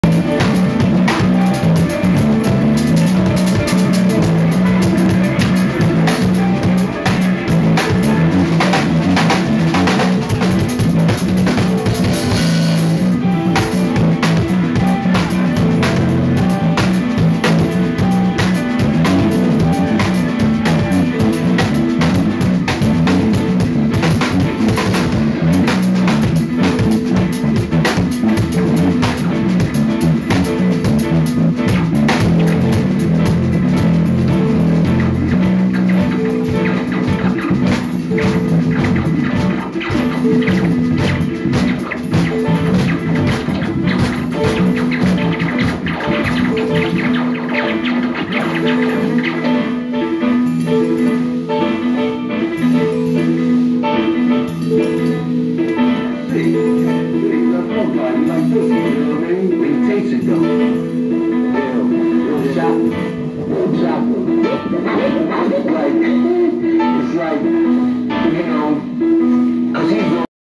Mark Guiliana BEAT MUSIC @ blue whale LA 1/24/2015, #1/5
Cole Whittle - bass
Troy Ziegler - electronics
David Binney - alto sax
Jason Lindner - keys